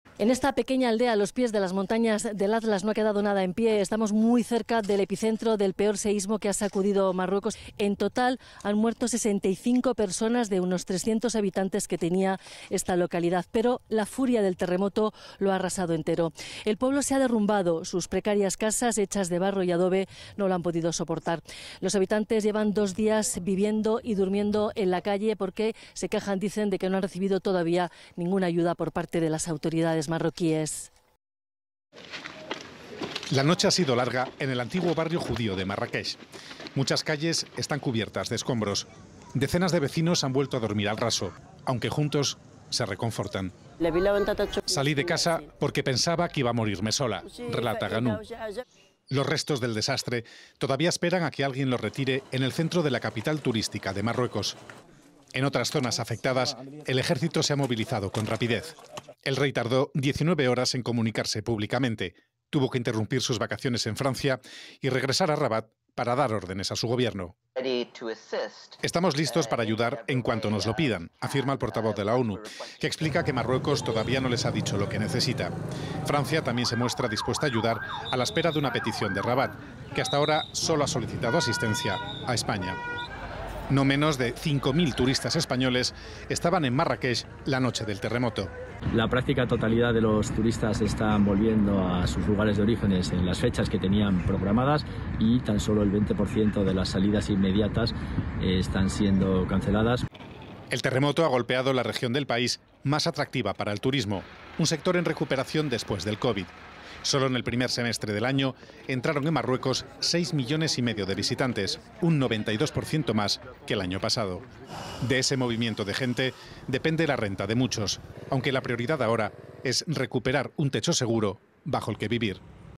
TERREMOTO MARRUECOS: SIGUE la BÚSQUEDA de SUPERVIVIENTES | RTVE Noticias
La segunda noche tras el terremoto en Marruecos ha sido larga en el antiguo barrio judío de la ciudad de Marrakech. Muchas calles están cubiertas de escombros y decenas de vecinos han vuelto a dormir al raso.

Los restos del desastre todavía esperan a que alguien los retire en el centro de la capital turística de Marruecos. En otras zonas afectadas, el Ejército se ha movilizado con rapidez.

El rey Mohamed VI tardó 19 horas en comunicarse públicamente, tuvo que interrumpir sus vacaciones en Francia para dar órdenes a su Gobierno. Cientos de turistas españoles estaban en Marrakech la noche del terremoto.